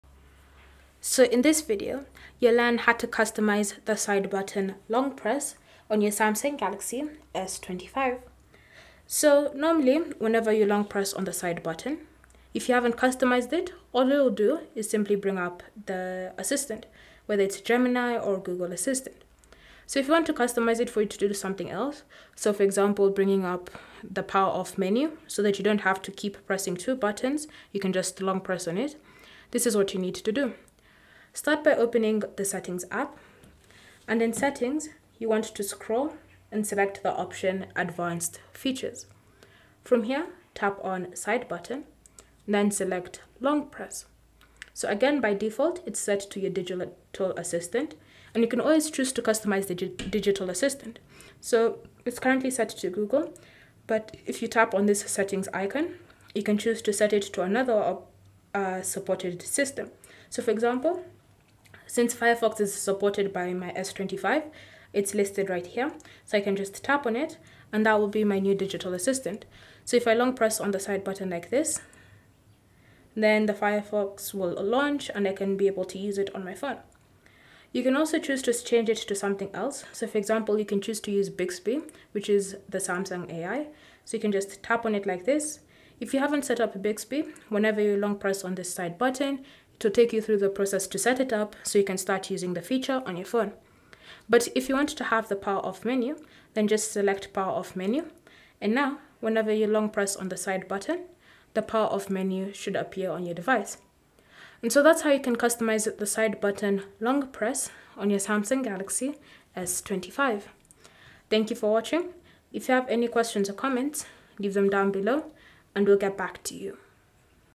How To Customize Side Button Long Press On Samsung Galaxy S25/S25 Ultra
Learn how to customize side button long press on Samsung Galaxy S25/S25 Ultra to quickly access your preferred functions.

By adjusting the side button long press on Samsung Galaxy S25/S25 Ultra, you can streamline your device’s operation and access features like the camera, apps, or other settings with just a press. Customizing the side button long press on Samsung Galaxy S25/S25 Ultra enhances your user experience and provides faster access to frequently used tools.

-Open the Settings app and go to "Advanced Features."
-Tap "Side Button" and then select "Long Press."
-By default, it's set to the digital assistant. To change it, tap the settings icon.
-Choose your preferred assistant or select "Power Off Menu" to bring up the power options.
-Now, a long press on the side button will trigger your selected option.

Cell Phone Tripod Adapter